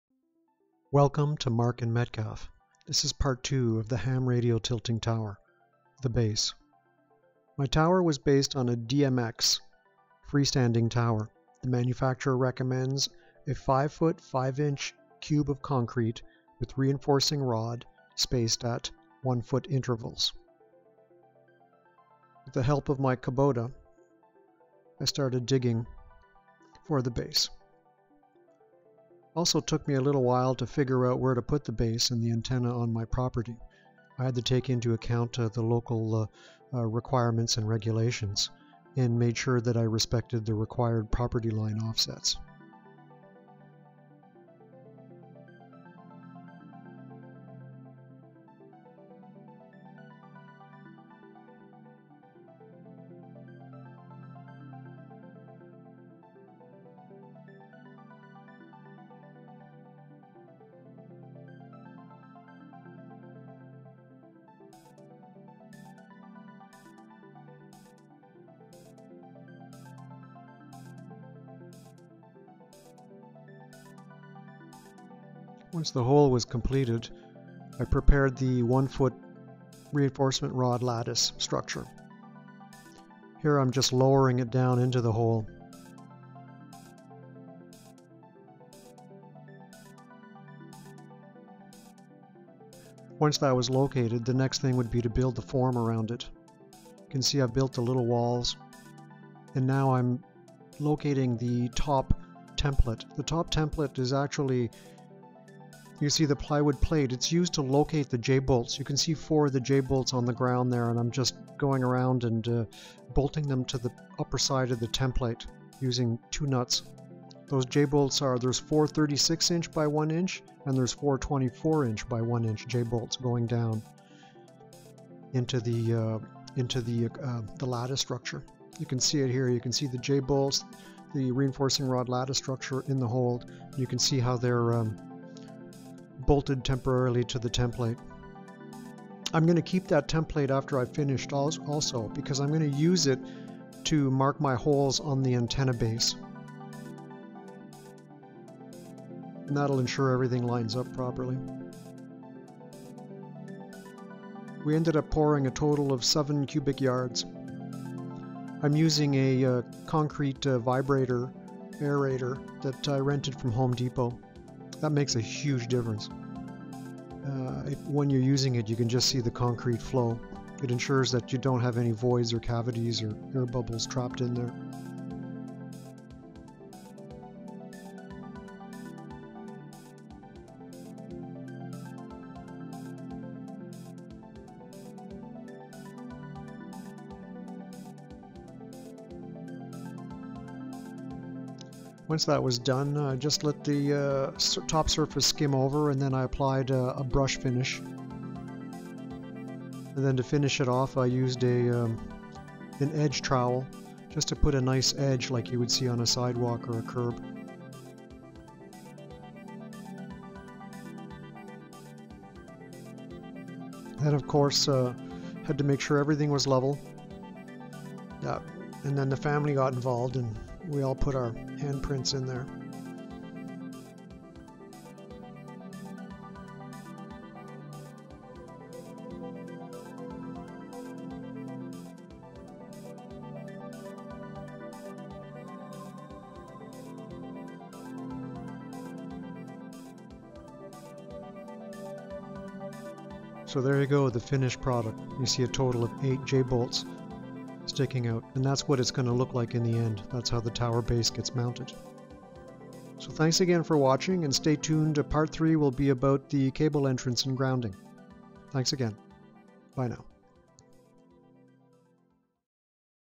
Tilting HAM Radio Antenna Tower Part 2 The Base
Here is a more detailed view of the tower base construction.
Additional videos will follow.